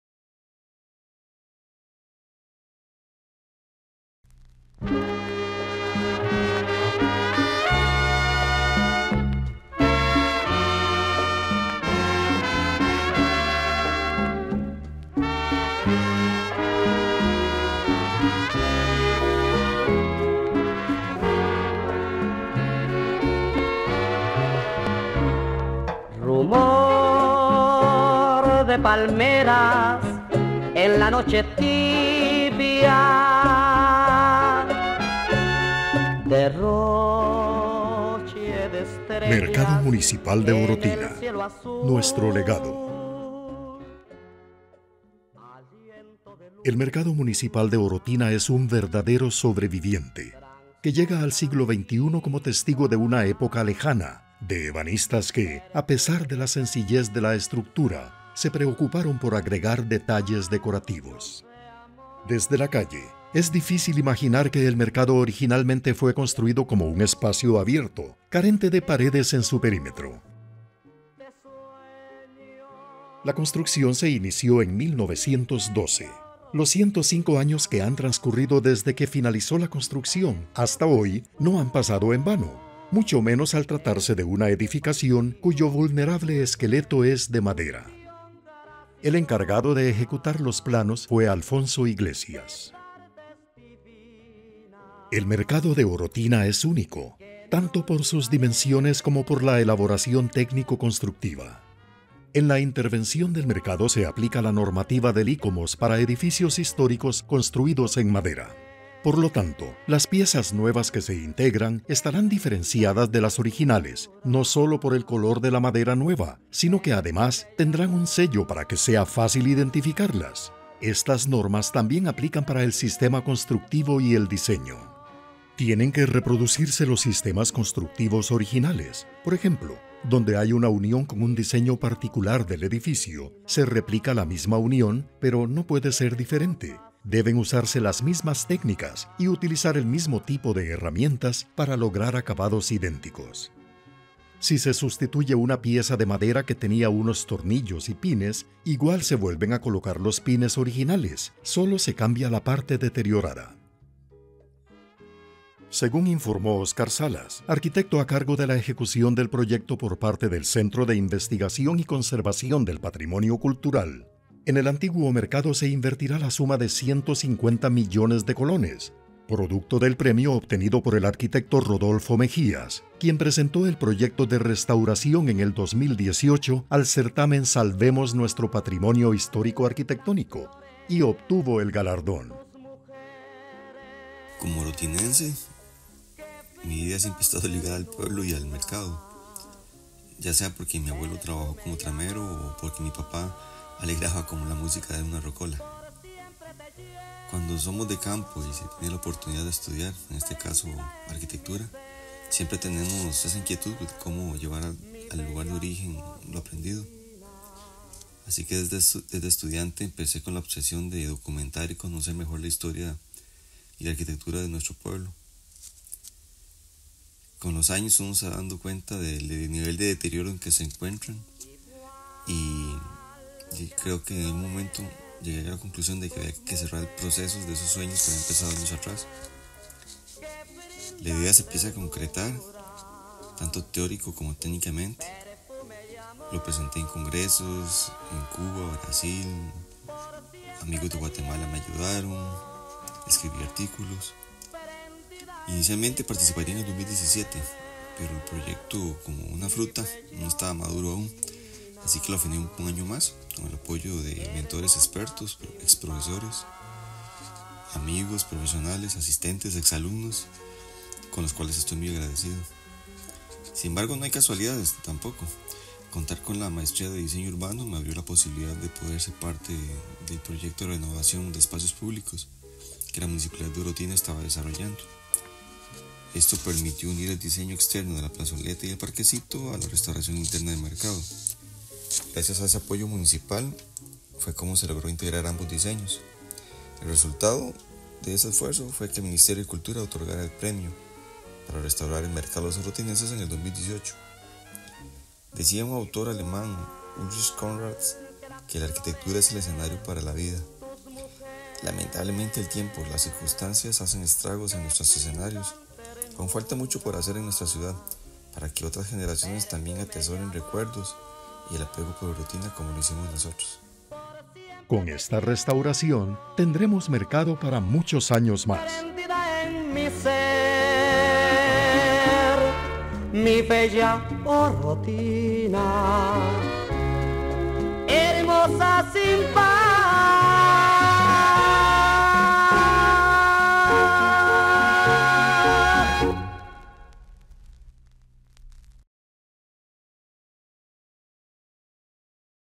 EL MERCADO MUNICIPAL DE OROTINA JOYA ARQUITECTÓNICA
El Mercado Municipal de Orotina es un verdadero sobreviviente que llega al siglo XXI como testigo de una época lejana.
Desde la calle, es difícil imaginar que el Mercado originalmente fue construido como un espacio abierto carente de paredes en su perímetro.
La construcción se inició en 1912;  los ciento cinco años que han transcurrido desde que finalizó la construcción hasta hoy no han pasado en vano, mucho menos al tratarse de una edificación cuyo vulnerable esqueleto es de madera.
El  mercado de Orotina es único, tanto por sus dimensiones como por la elaboración técnico-constructiva